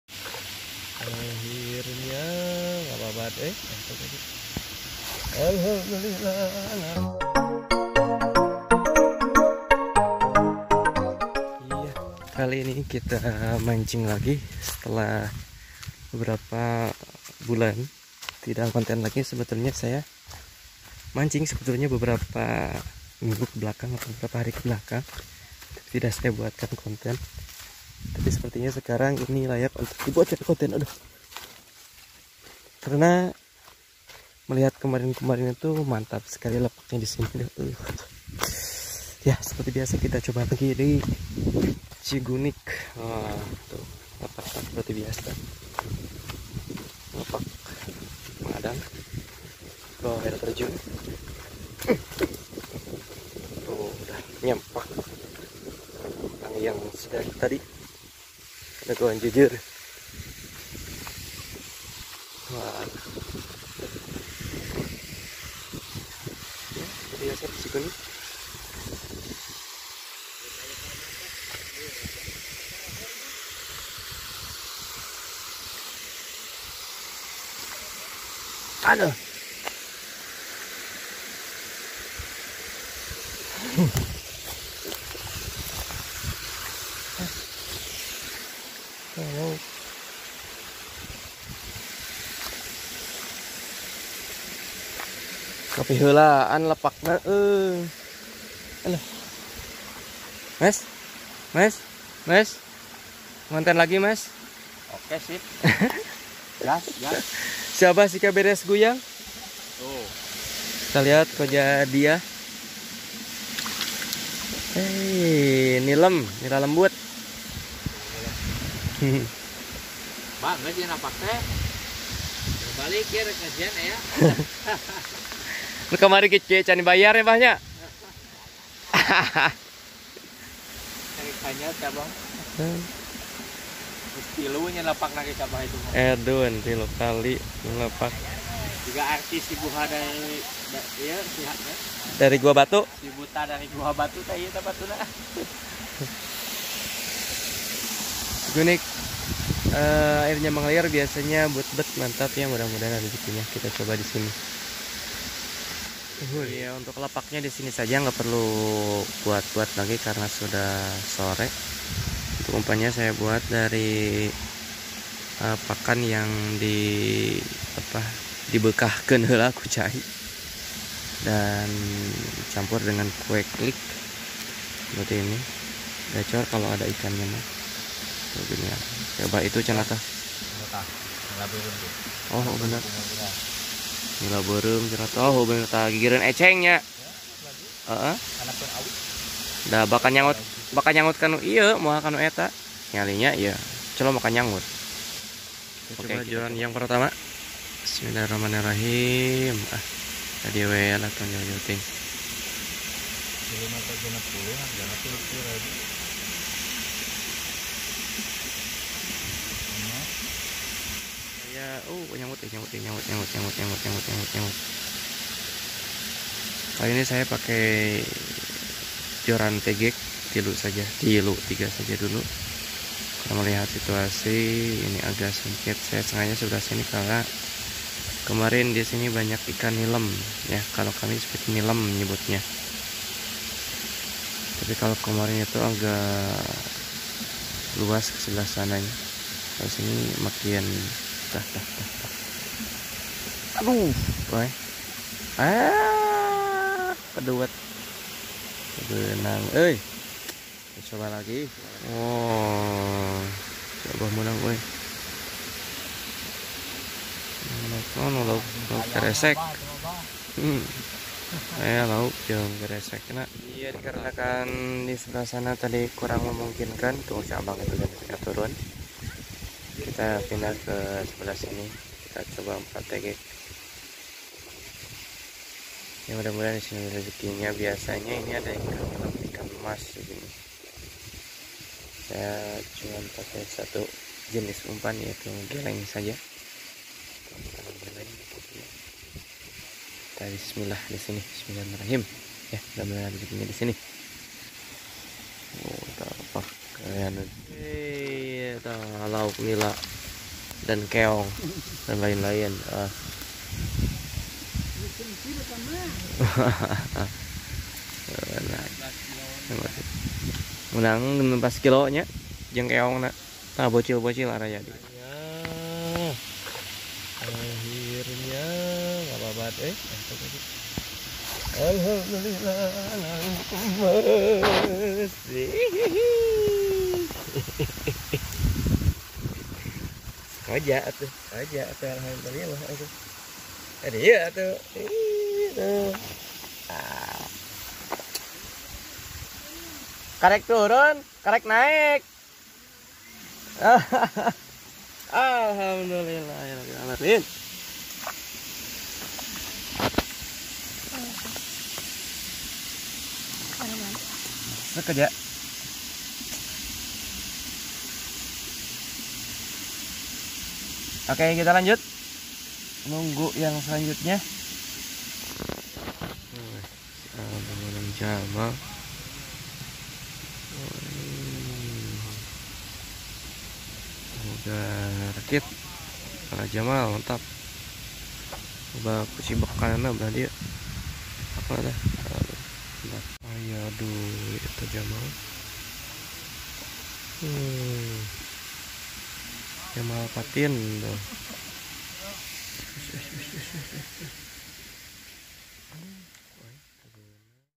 Mancing Cirata, Kembali Ke Lapak Cigunik Setelah Tujuh Purnama Berlalu | SHM Eps. 50
Selamat bergabung kembali untuk semua wargi Sarjana Hobi Mancing. 🥳
Mohon maaf baru bisa ngonten lagi dikarenakan semangat saya kemarin hilang akibat hal yang tidak terduga. Semoga ke depannya saya lebih giat lagi dalam membuat konten hiburan mancing untuk semua.